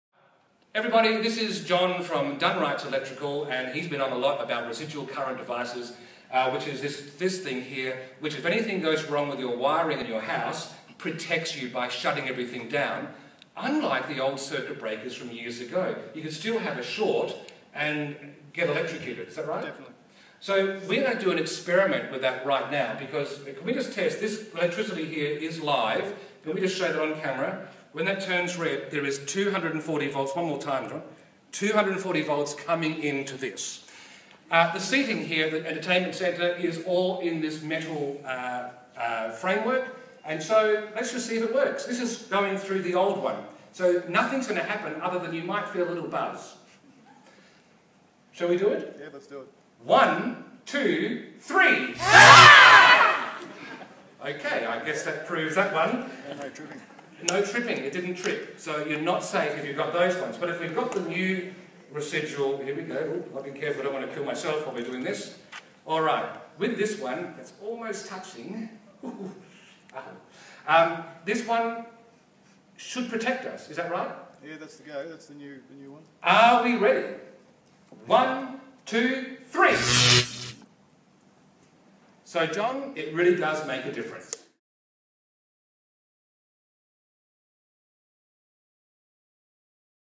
Dunwrights Electrical Demonstration of RCD Residual Current
Jon Storey from Dunwrights Electrical, Darwin, demonstrates the RCD in front of a live audience